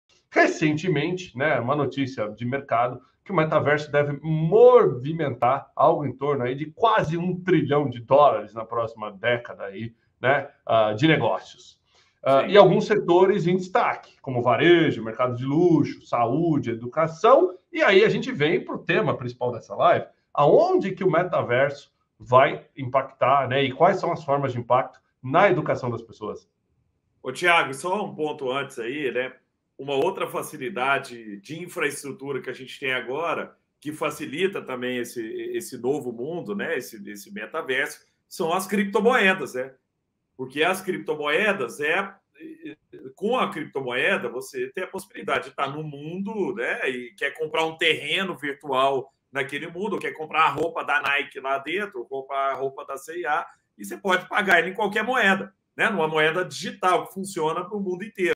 Entrevista pela Forbes - Tiago Alves
Entrevista de um assunto bem atual: O metaverso e seus impactos no futuro da educação!

Entrevista inédita pela @forbesbr com o Fundador da  @_sambatech @_sambadigital com o queridíssimo @gustavocaetano

Live completa no Canal da Forbes Brasil no Youtube!